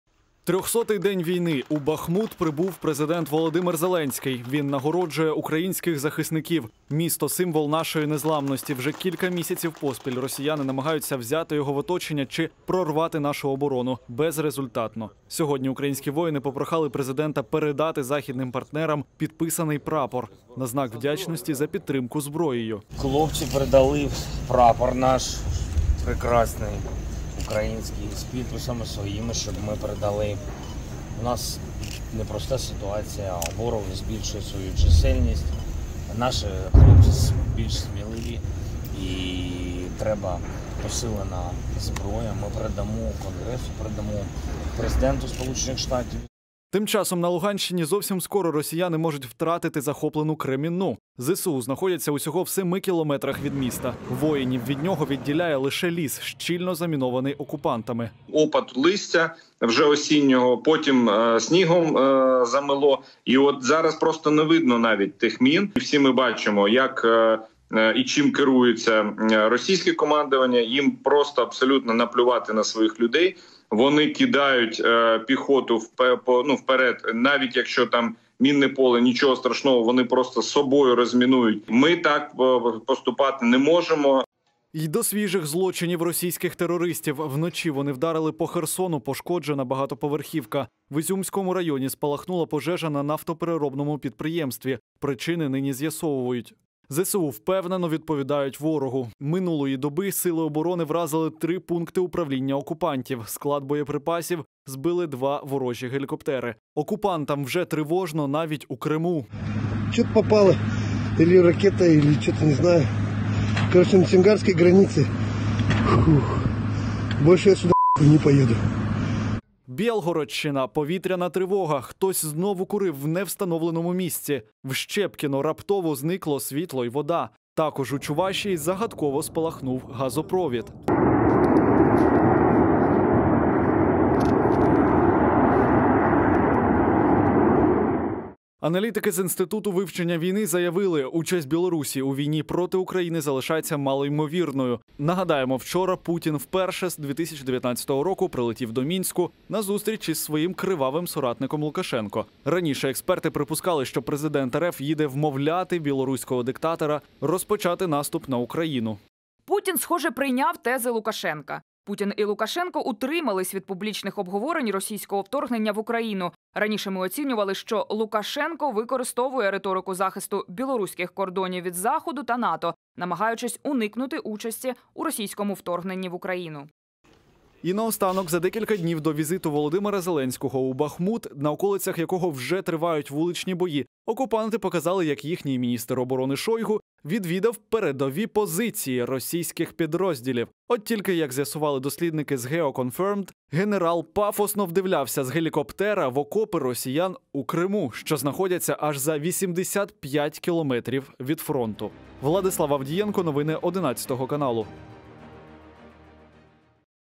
Зеленський нагородив військових у Бахмуті. ЗСУ підходять до Кремінної. 300 доба на фронті
300 днів української незламності! Майже 10 місяців Україна виборює своє право на суверенітет та територіальну цілісність у війні з російськими загарбниками. В найгарячішу точку фронту - Бахмут - сьогодні прибув президент України Володимир Зеленський. На Бєлгородщині - знову бавовна. ЗСУ до окупованої Кремінної - лишився останній ривок. Також аналітики оцінили ризики повноцінного вступу у війну Білорусі. Про усе це детальніше - далі в сюжеті.